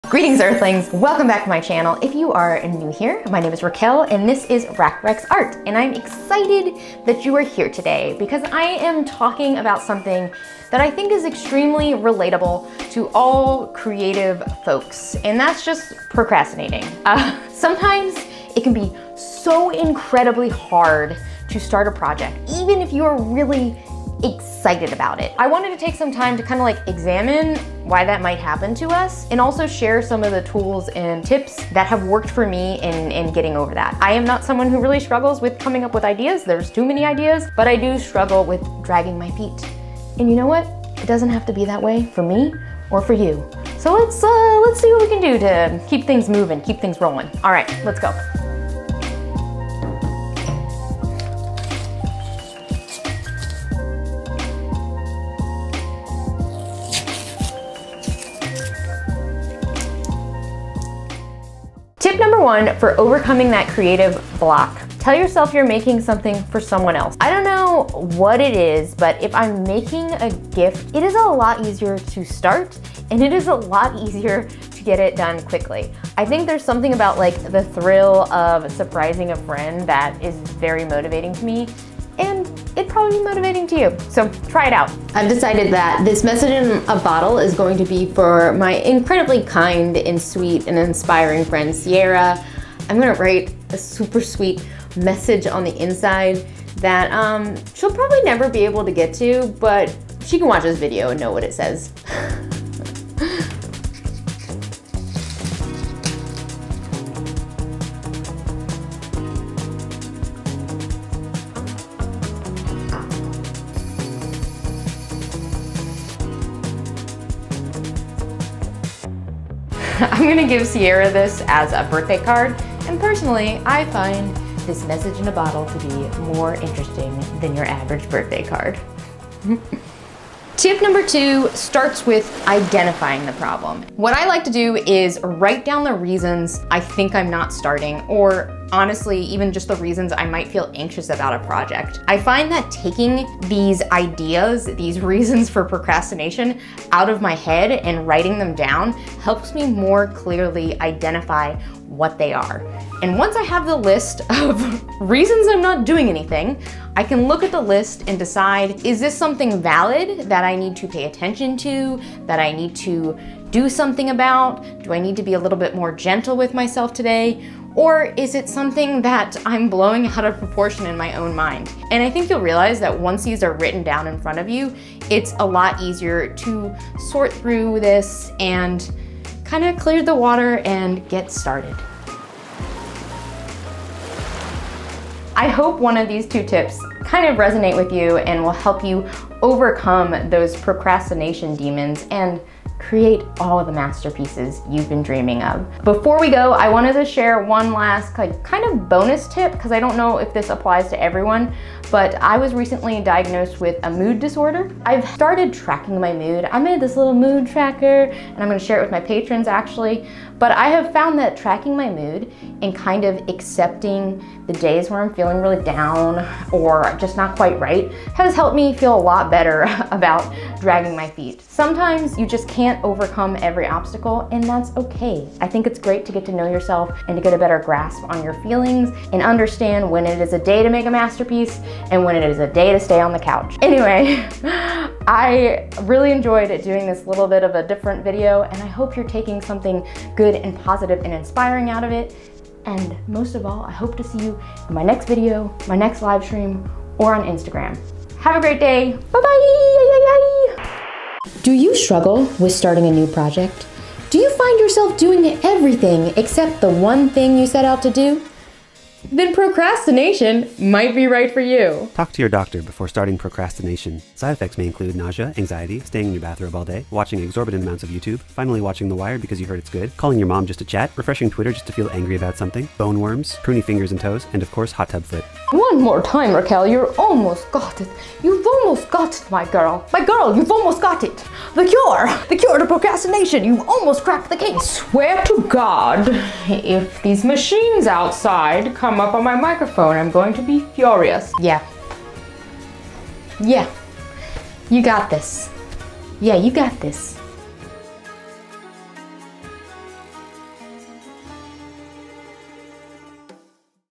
How to STOP Procrastinating 🎨 Quick Tips for Artists
A big thanks to @StudsonStudio for the amazing voice over cameo 😂
This video is a bit different but I wanted to share some creative tips that have helped me to get moving! Even though I love painting minis, sculpting, and making fun crafts it sometimes can be hard to get started. Here's hoping 2022 brings us all inspiration & artistic productivity while leaving us enough brain space to relax and still enjoy the process ✨

If you try it out with the link below we’ll both get some free time to use their music 😎